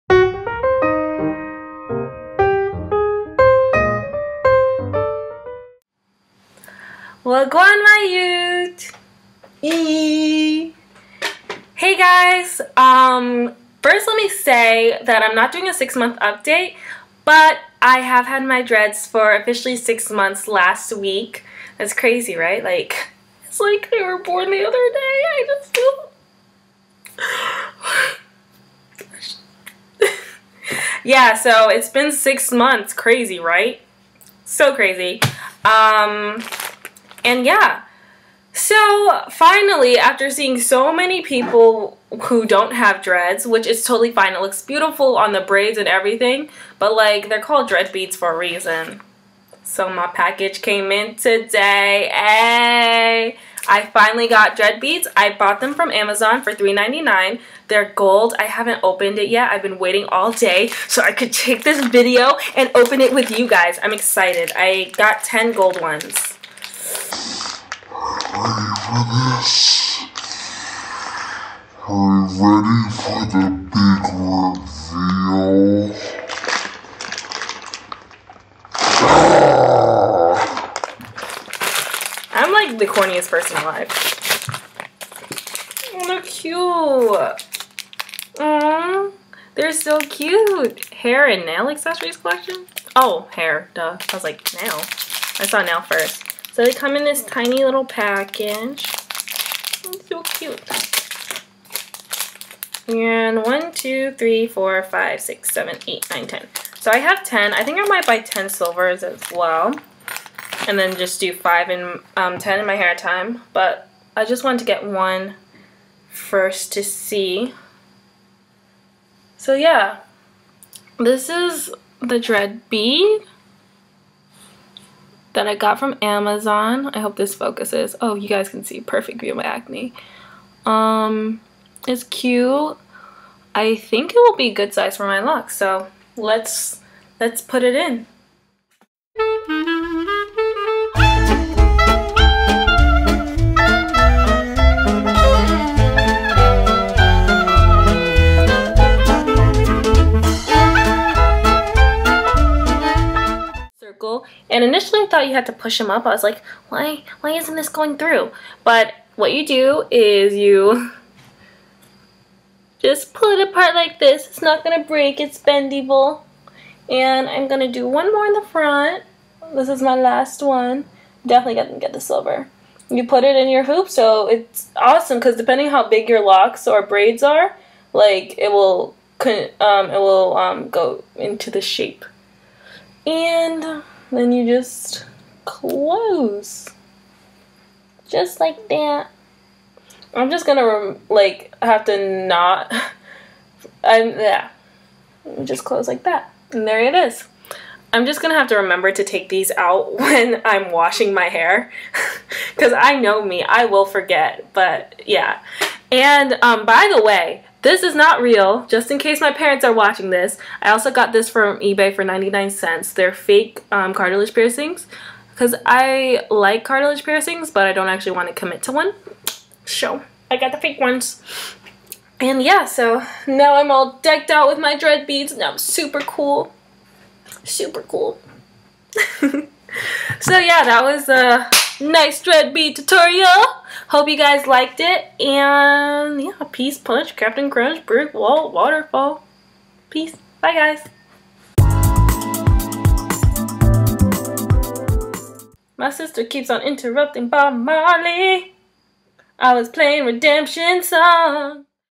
Dread Beads
I finally got dread beads! This is just a dread bead tutorial.

Link to the dread beads